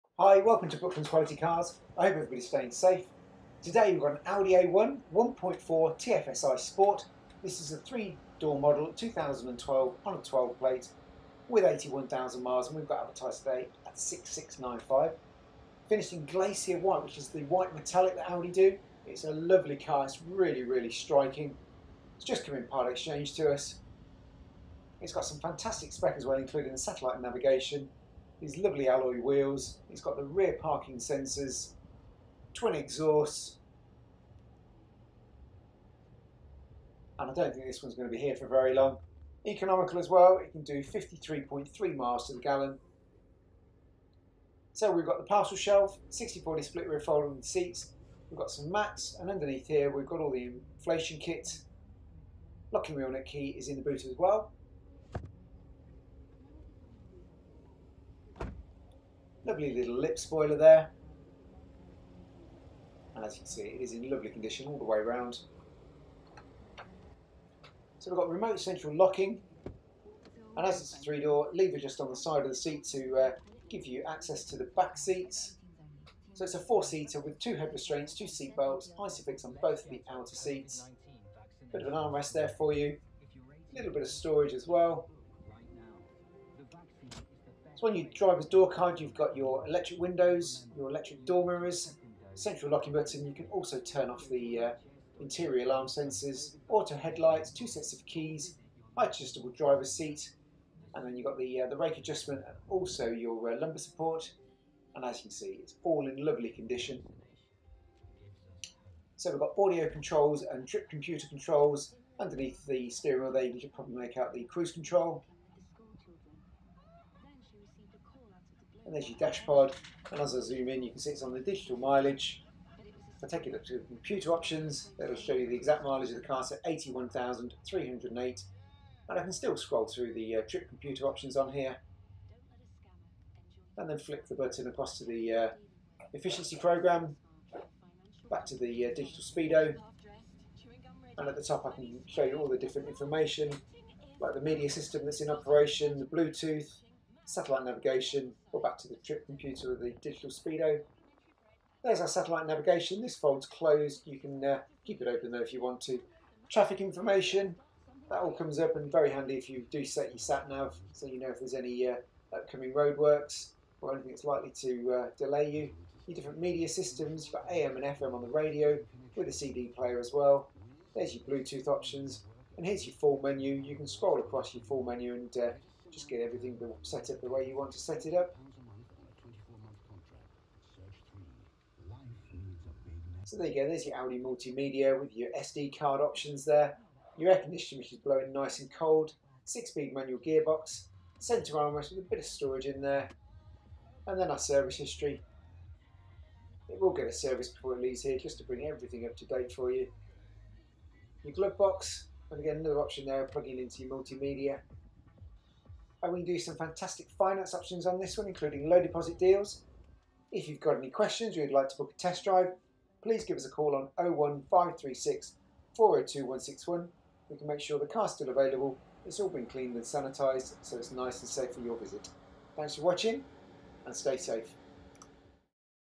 Brooklands Quality Cars Audi A1 Sport 1.4TFSi
Audi A1 Sport 1.4TFSi 2012 12 finished in Glacier White with 81308 miles Great Service History @6695